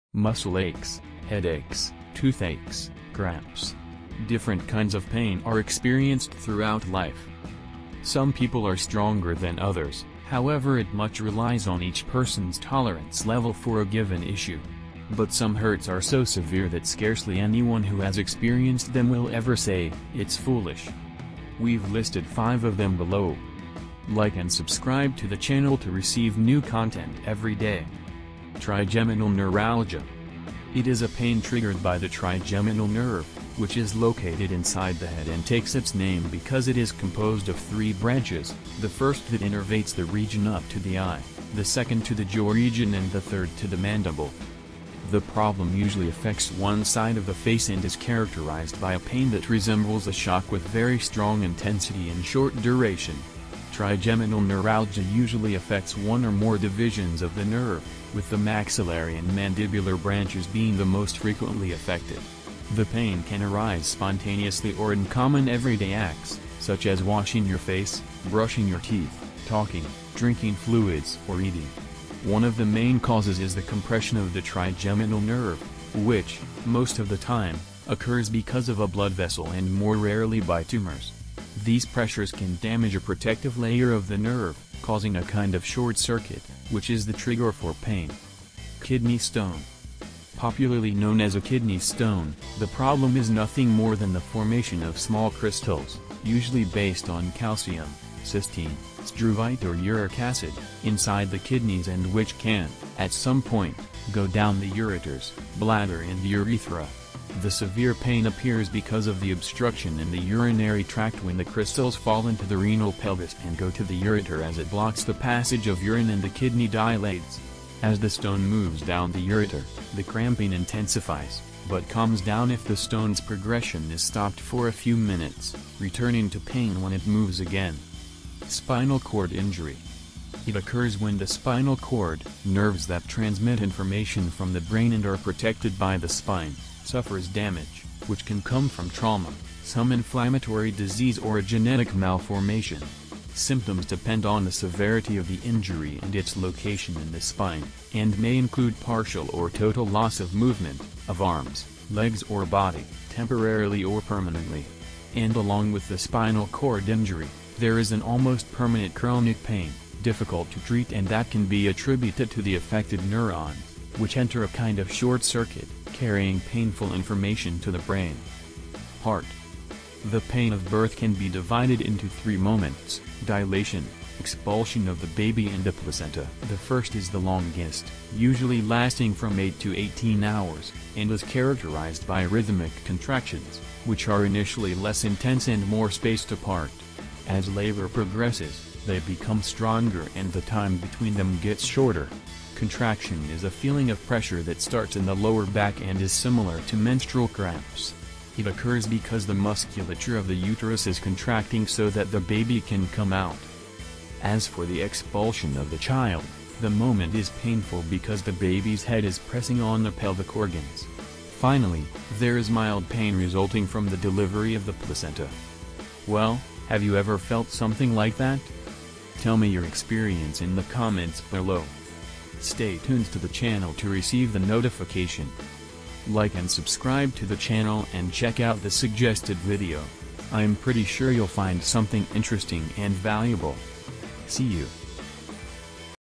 These are the most painful things that can happen to you at any time
It occurs when the spinal cord - nerves that transmit information from the brain and are protected by the spine - suffers damage, which can come from trauma, some inflammatory disease or a genetic malformation.

Part

The pain of birth can be divided into three moments: dilation, expulsion of the baby and the placenta.

Trigeminal neuralgia

It is a pain triggered by the trigeminal nerve